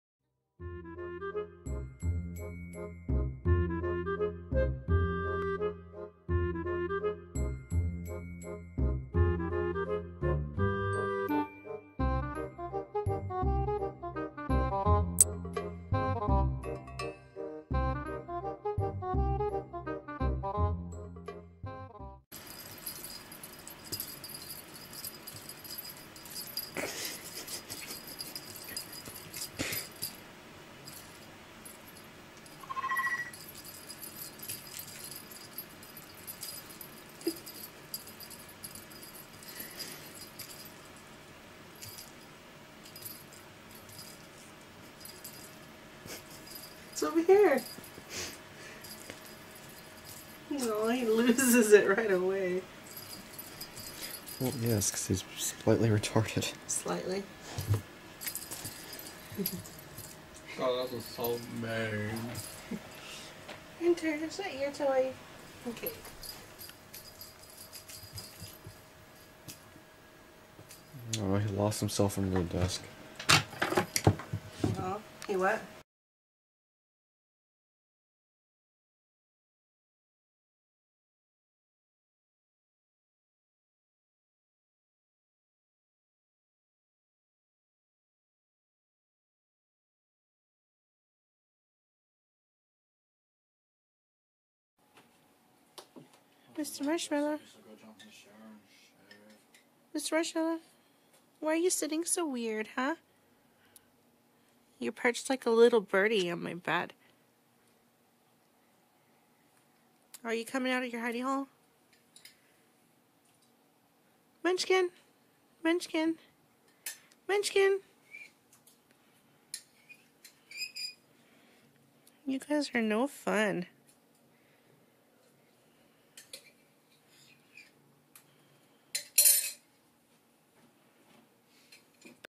Mr. Marshmallow playing (kitten with neurological issues)
With the impending birth of Marmalade's babies and people wanting to see more of Mr. Marshmallow, I thought I'd take the time to upload old videos of past and current pets. None of these kittens are from Marmalade! She has not given birth yet. In these videos will be dogs and cats I've had over the last 9 years or so. You'll know how old the videos are by my weight haha! I will be uploading one video of pets a day (alongside my regularly scheduled videos), so if you aren't an animal lover feel free to skip these.